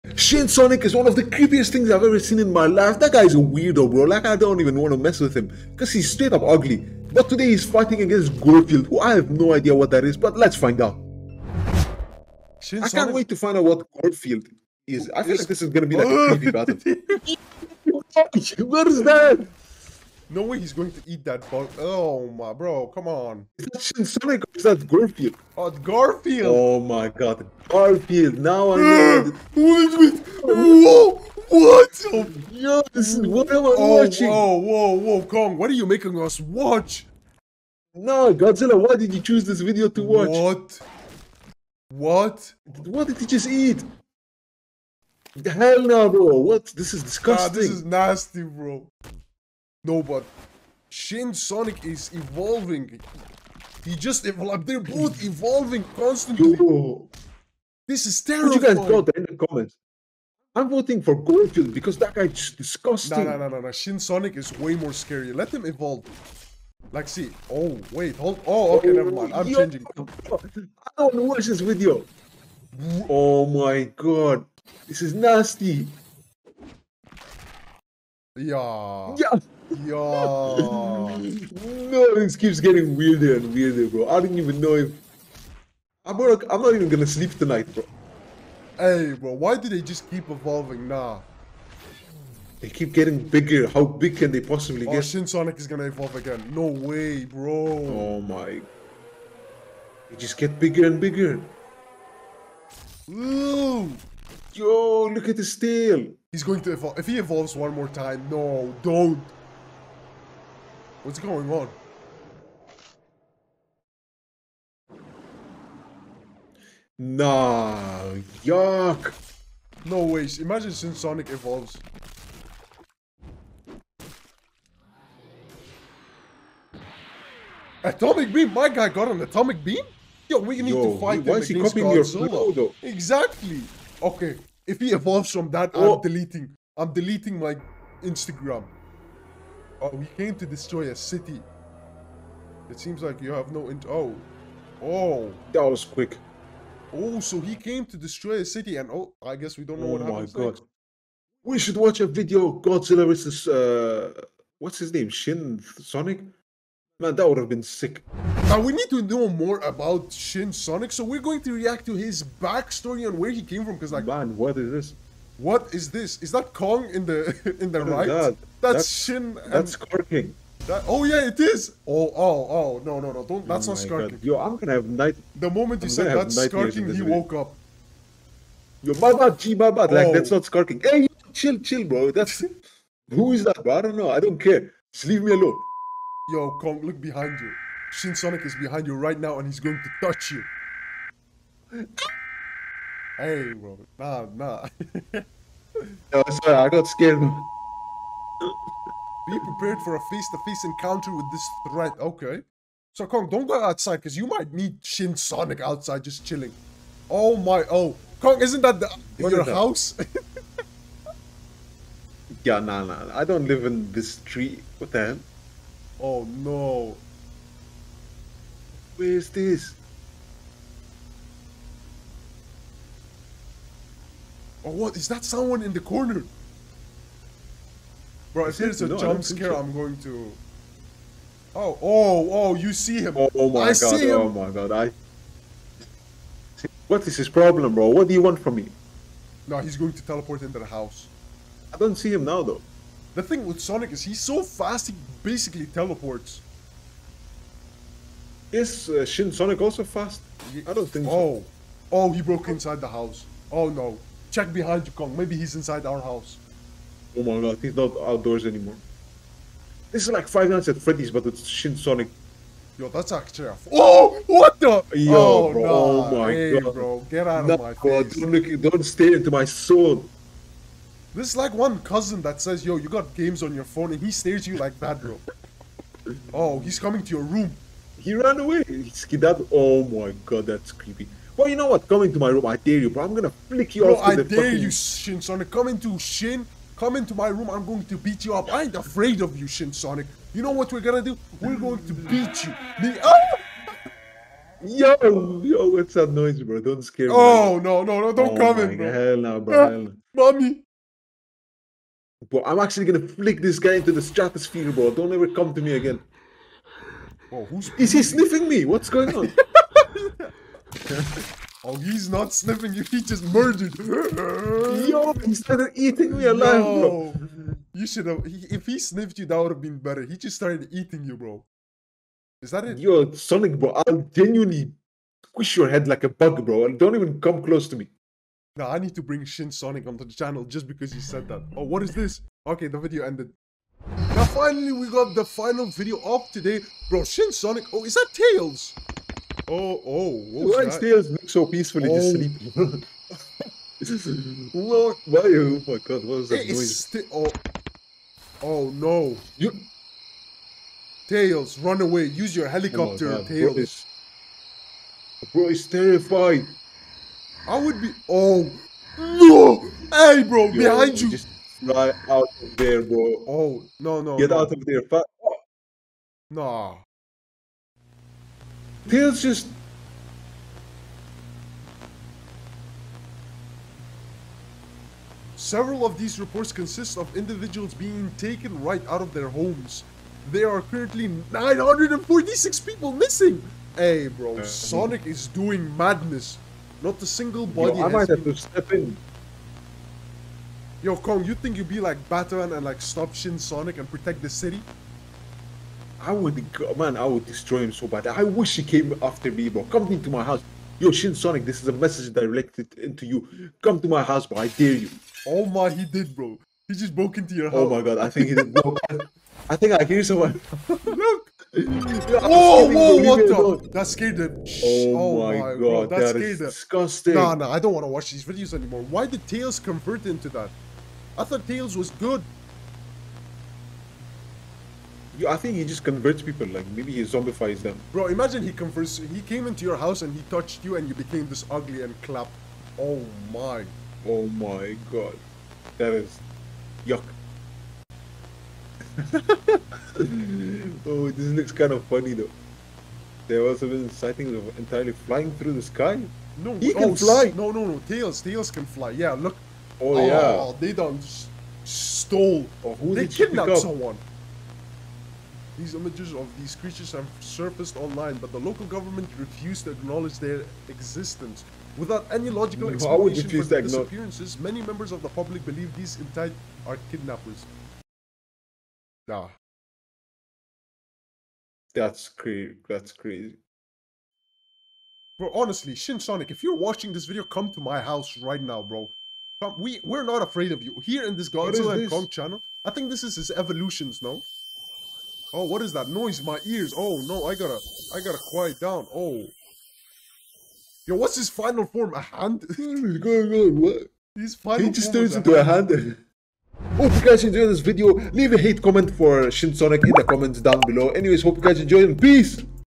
Shin Sonic Tapes vs Gorefield INSANE Reaction
Today we react to godzilla and kong roast battle

Original Video:  @MinHp

Original Video: @Evanimas2